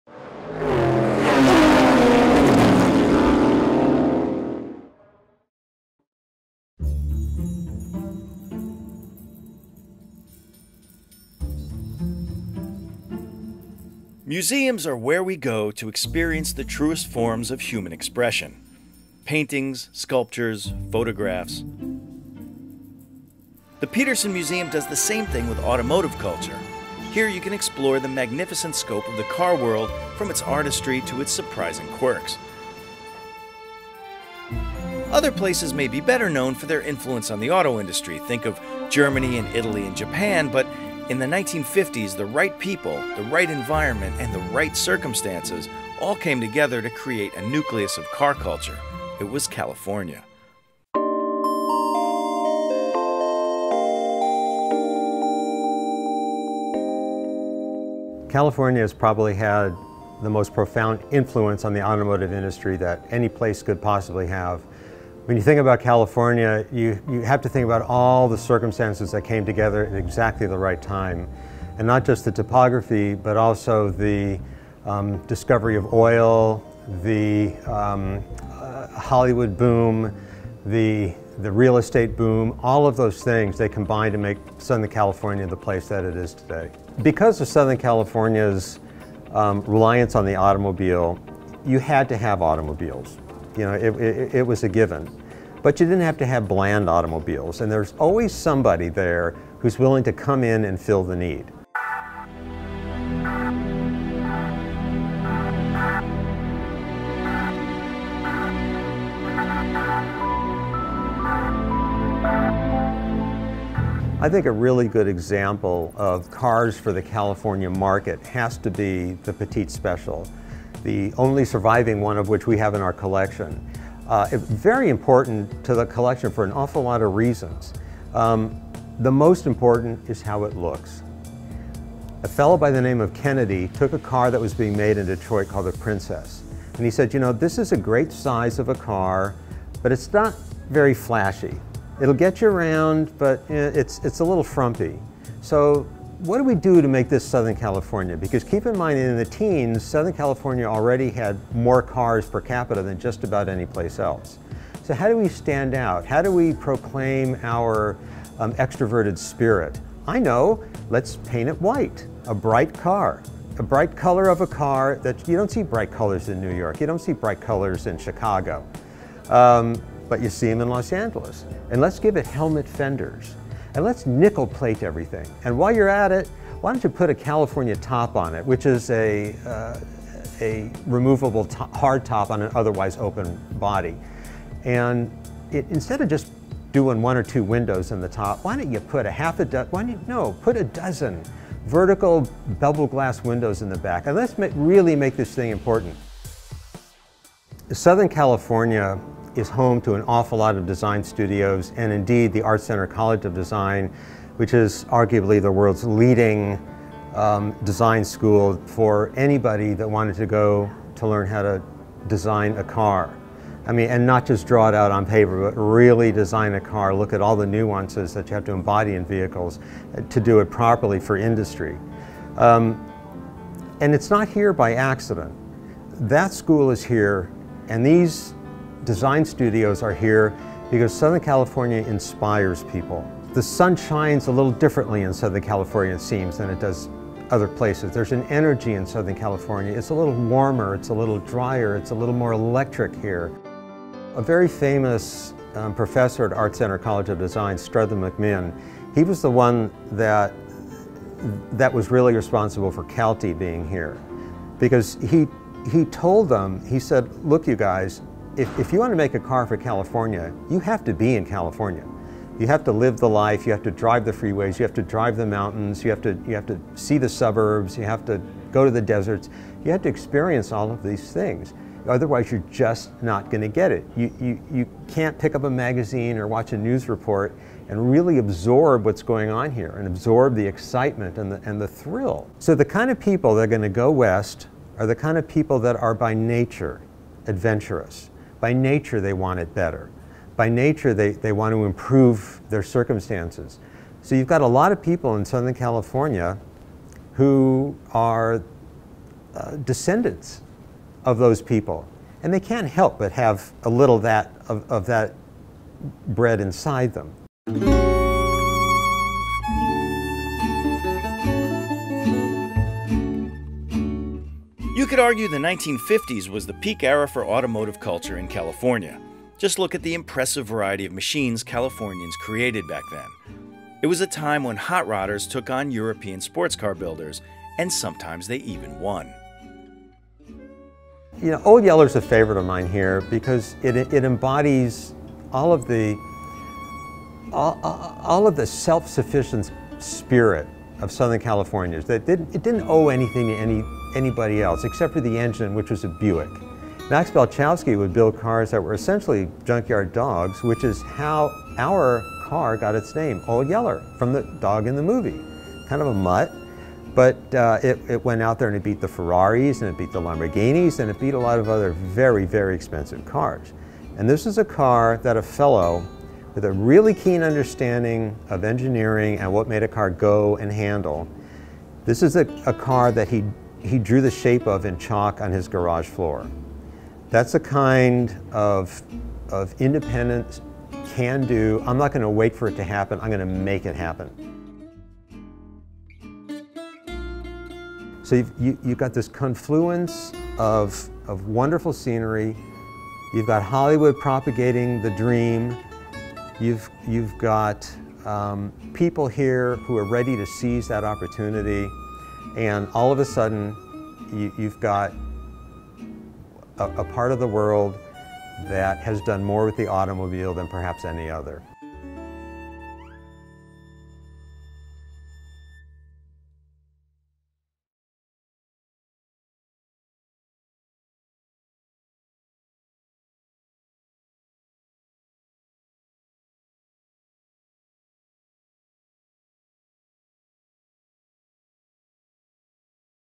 Here's Why California Is The Center Of Car Culture
In part one of our California Car Culture series:  What is it about California that's made it such an important place in car culture? We ask expert Leslie Kendall, curator of the Petersen Museum in Los Angeles to give us the inside dope.